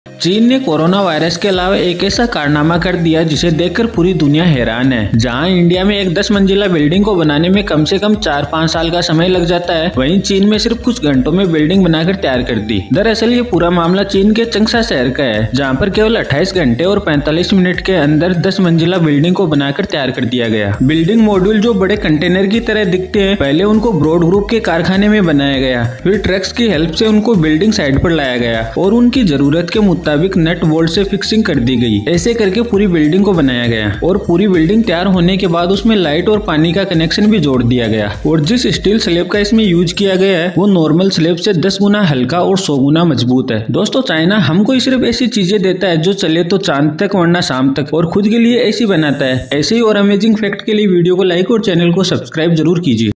china made building in 28 hours | broad group china
10 storey building constructed in 28 hours and 45 mins | broad group china ||